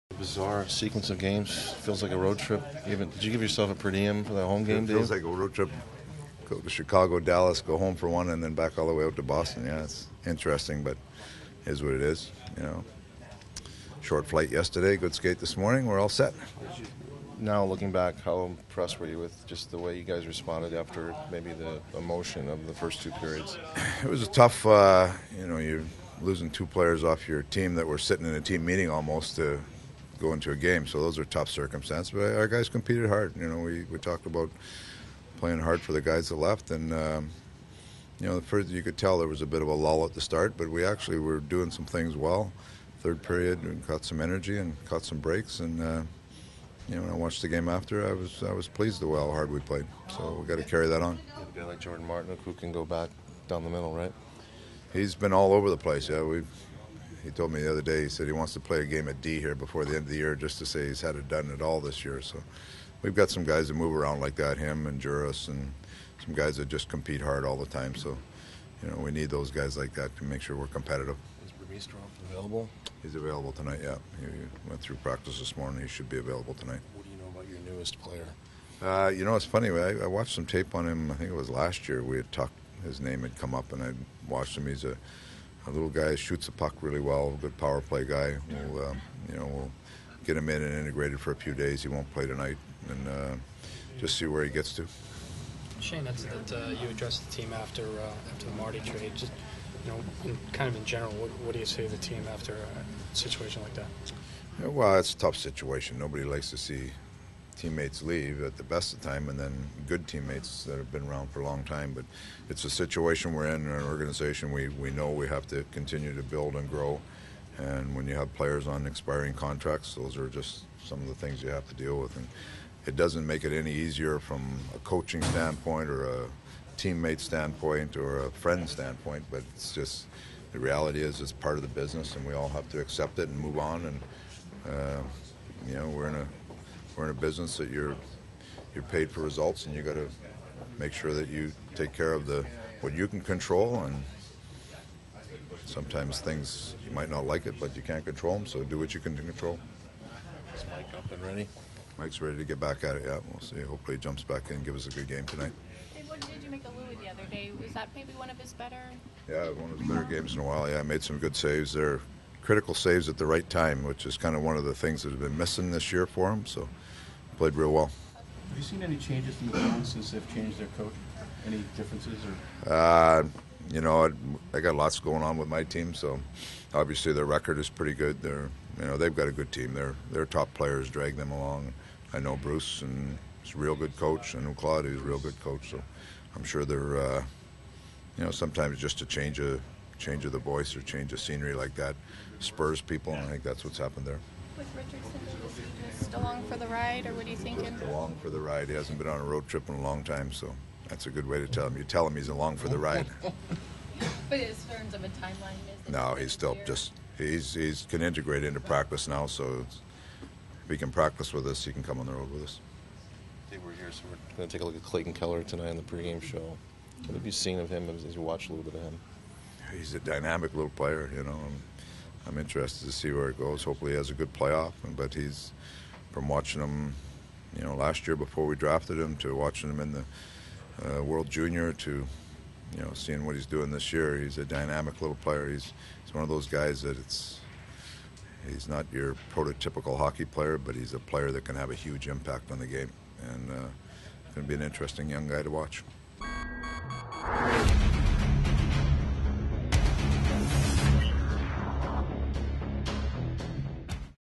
02/28/17: Dave Tippett Q&A
From the morning skate in Boston, Coyotes Head Coach Dave Tippett offers pre-game insight into Tuesday's game vs. the Bruins.